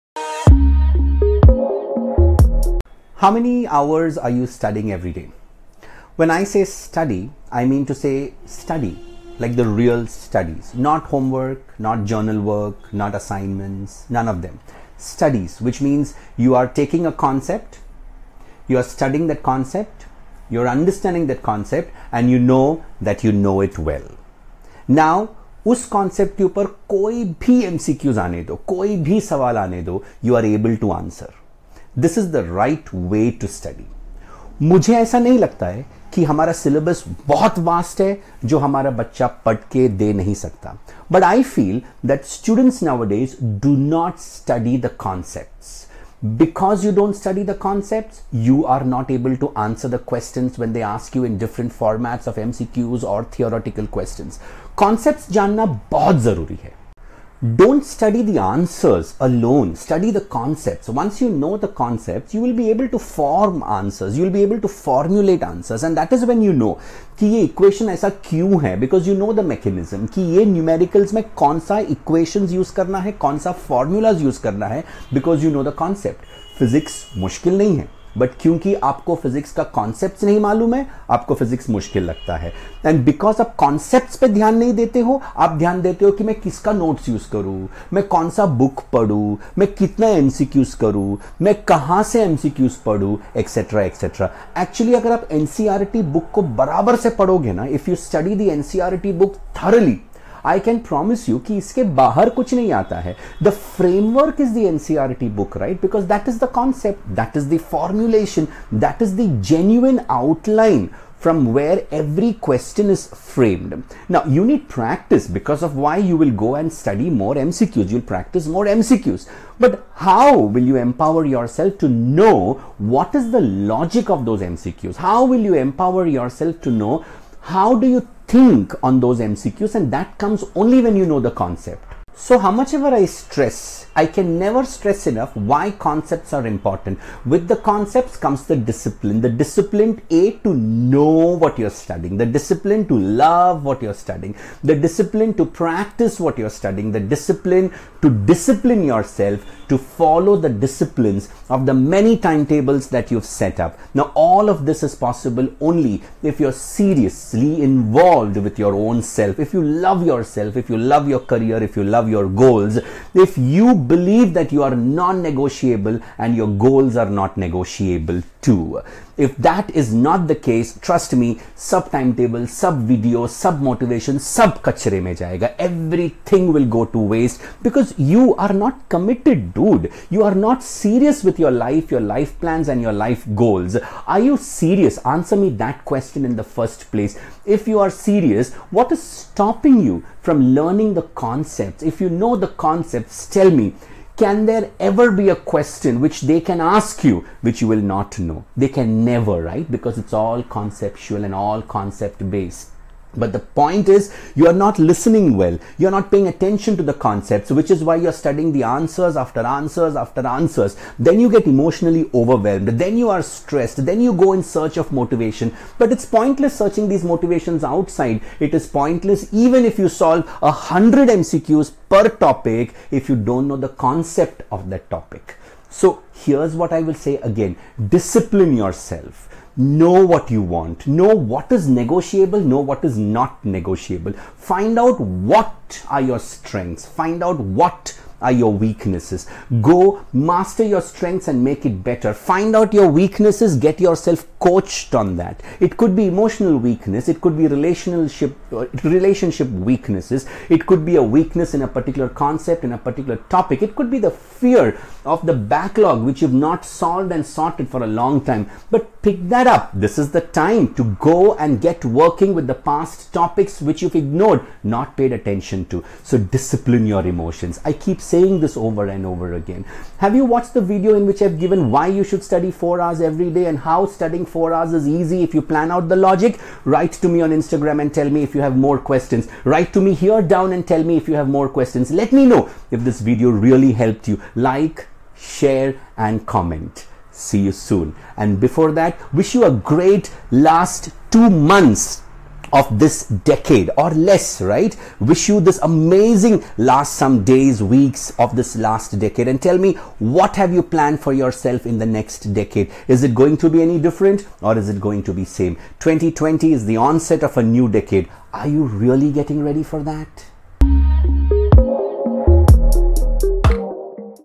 Prepare for NEET 2020 by learning the concepts
Concepts are important !

Its pointless if you work hard on MCQs or Q and A without understanding the concepts.

in less than 70 days you will be entering into the New decade, how are you planning to fulfill your Goals in 2020
cheers to all that you wish for yourself and extra.